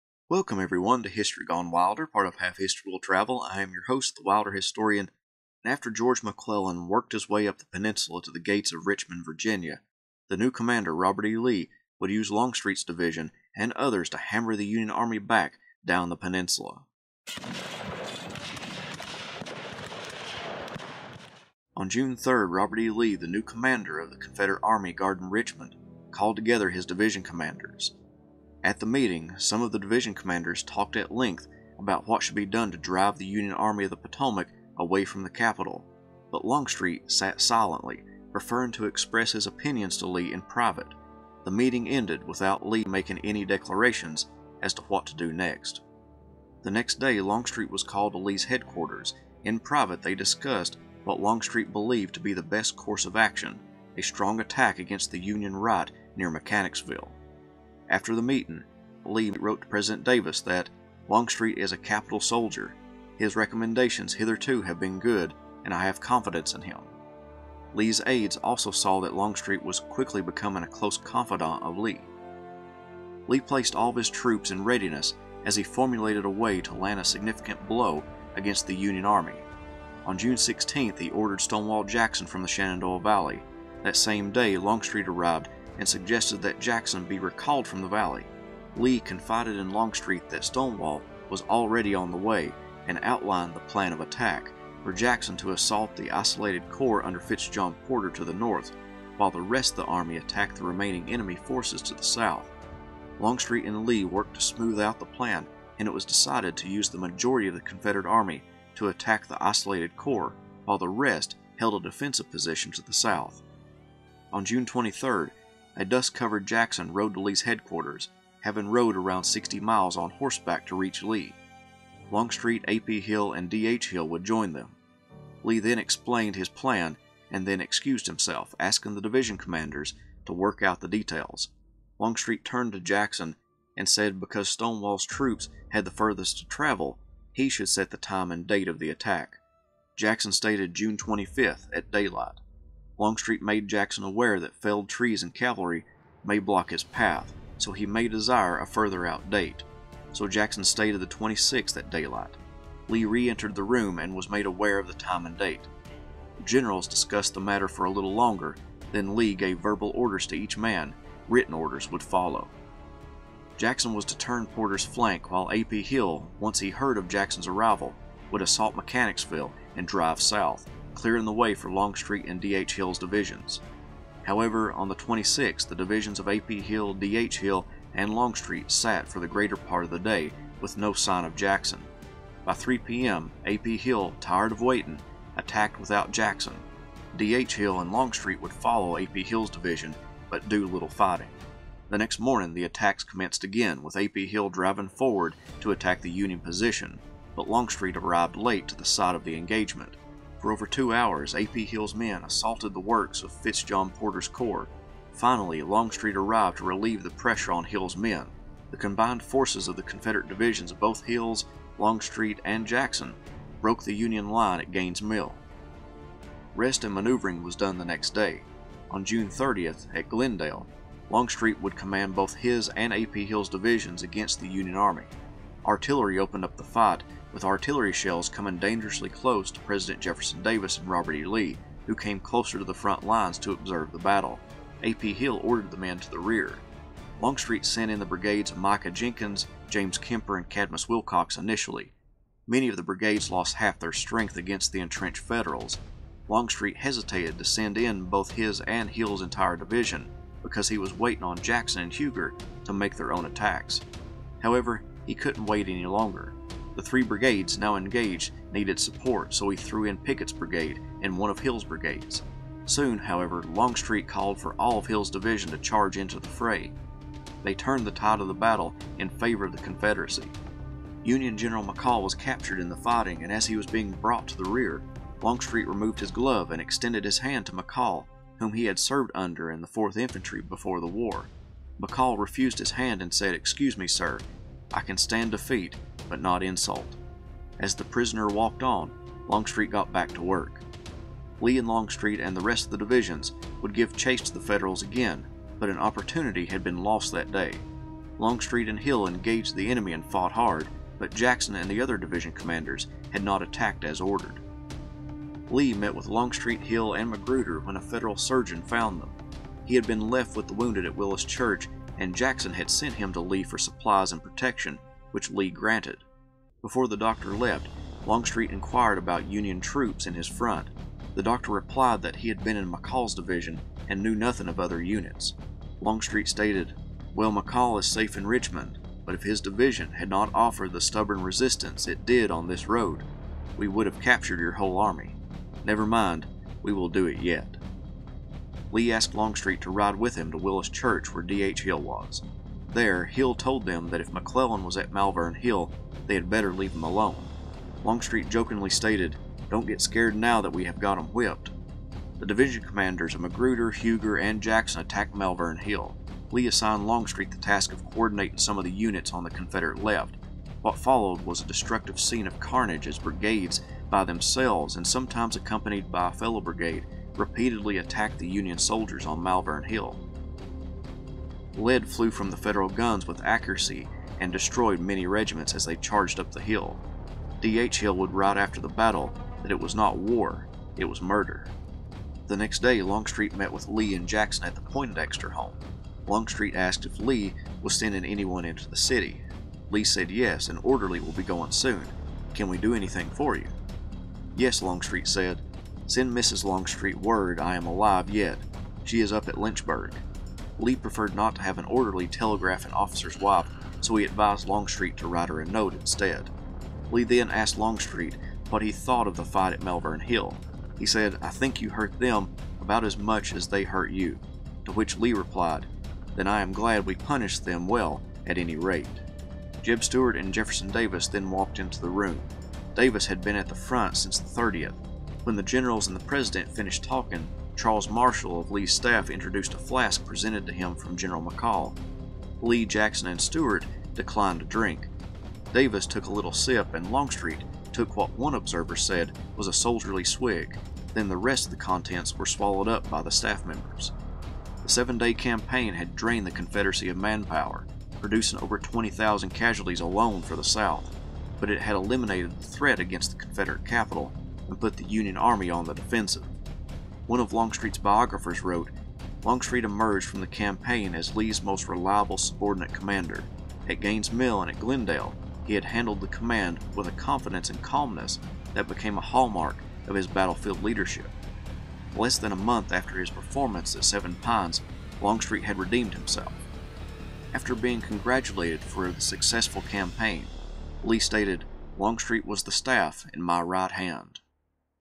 James Longstreet: Seven Days | Part 8
Confederate Lieutenant General James Longstreet is one of the most controversial figures of the American Civil War. His political views after the war turned a lot of his former comrades against him. Critics attacked him for his choices at the Battle of Gettysburg, his absence at Chancellorsville, and many other military aspects. In this biography, I will highlight the life of this most interesting commander from the Civil War and illuminate his private and public life. In this episode, we learn about Longstreet's role in the Seven Days.